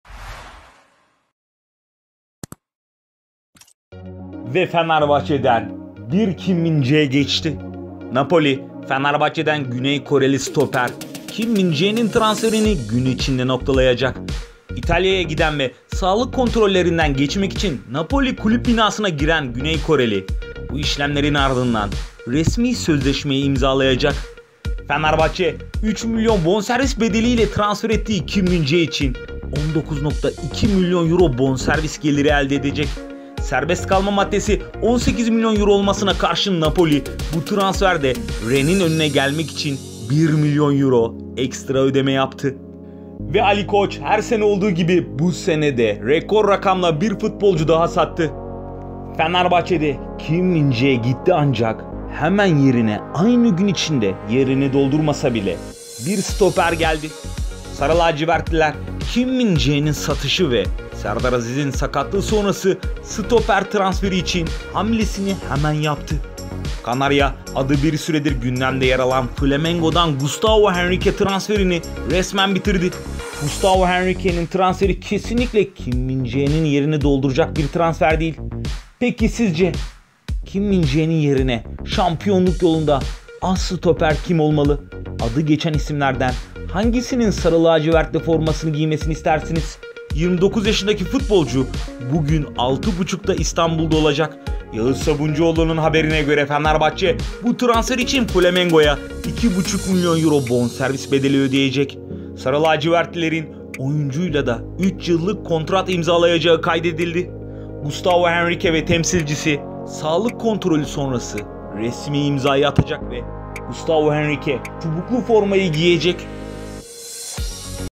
Gustavo Henrique geldi Kim de Tesislere girdi ! 8. Bomba F.Bahçe'de...
Napoli, Fenerbahçe'den Güney Koreli stoper Kim Min-Jae'nin transferini gün içerisinde noktalayacak. İtalya'ya giden ve sağlık kontrollerinden geçmek için Napoli kulüp binasına giren Güney Koreli, bu işlemlerin ardından resmi sözleşmeyi imzalayacak.

Fenerbahçe, 3 milyon euro bonservis bedeliyle transfer ettiği Kim Min-Jae için 19.2 milyon euro bonservis geliri elde edecek. Serbest kalma maddesi 18 milyon euro olmasına karşın Napoli, bu transferde Rennes'in önüne geçmek için 1 milyon euro ekstra ödeme yaptı.

Fenerbahçe, Kim Min Jae'nin satışı ve Serdar Aziz'in sakatlığı sonrası stoper transferi için hamlesini yaptı.

Sarı-lacivertliler, adı bir süredir gündemde yer alan Flamengo'dan Gustavo Henrique transferini bitirdi. 29 yaşındaki futbolcu, bugün 18.30'da İstanbul'da olacak.

Yağız Sabuncuoğlu'nun haberine göre; Fenerbahçe, bu transfer için Flamengo'ya 2.5 milyon euro bonservis bedeli ödeyecek. Sarı-lacivertlilerin, oyuncuyla da 3 yıllık kontrat imzalayacağı kaydedildi.

Gustavo Henrique ve Temsilcisi Luis Augusto sağlık kontrolü sonrası resmi imzayı atacak.